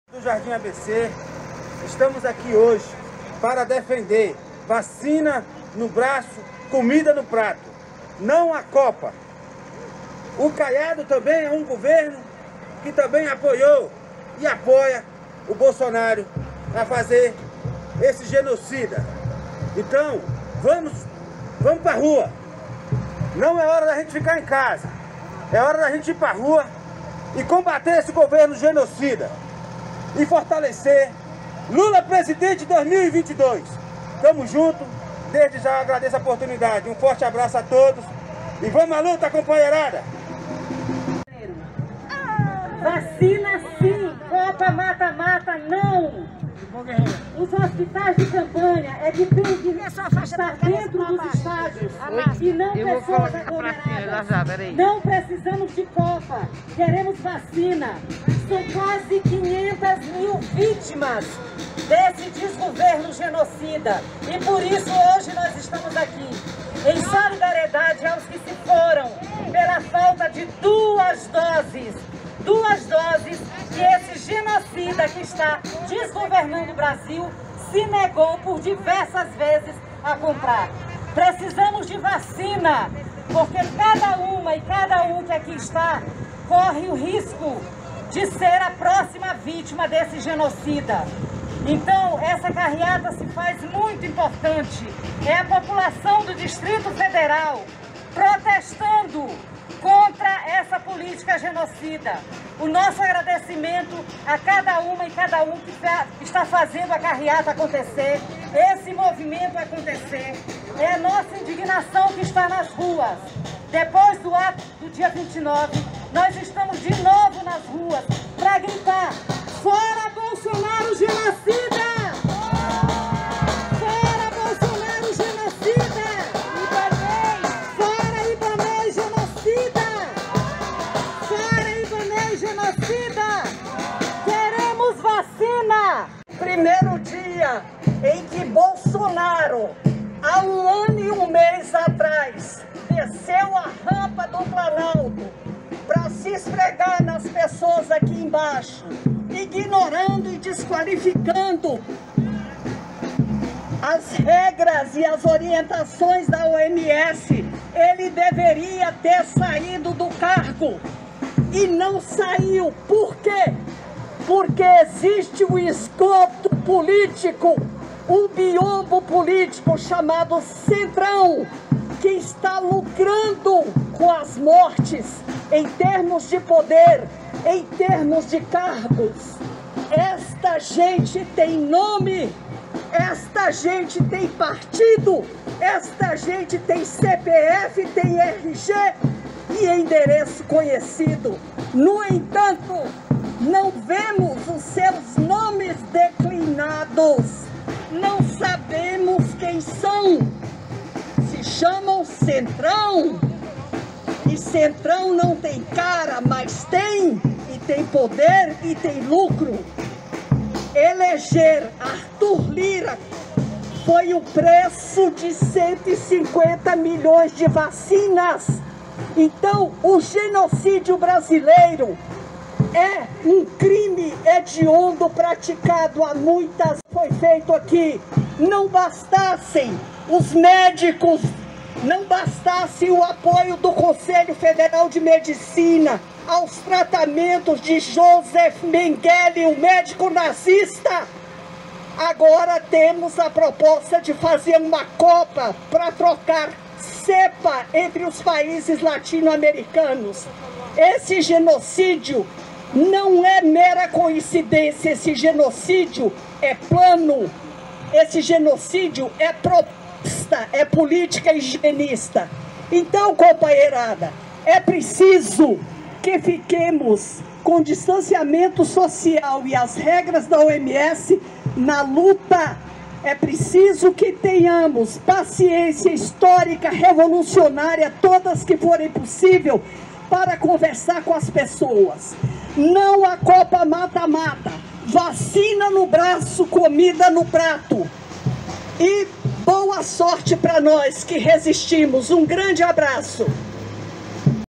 CARREATA EM BRASÍLIA PELA VACINA CONTRA A COPA (6/6/2021)
Brasília amanheceu com povo nas ruas, milhares de manifestantes saíram em carreata para defender "vacina no braço, comida no prato".
IMAGENS: Marilene Lara